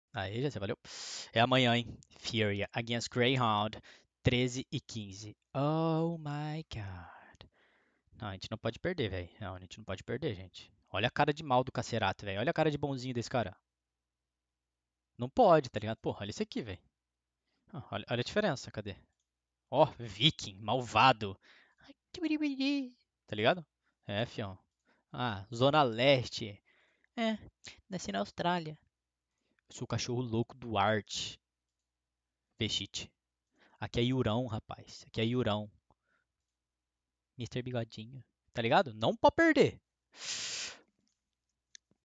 HONDA MOSTRANDO PQ A FURIA NÃO PODE PERDER PARA GRAYHOUND! KKKKKKKKKKKKKKKKKKKKKKKKKK
HONDA MOSTRANDO PQ A FURIA NÃO PODE PERDER PARA GRAYHOUND! KKKKKKKKKKKKKKKKKKKK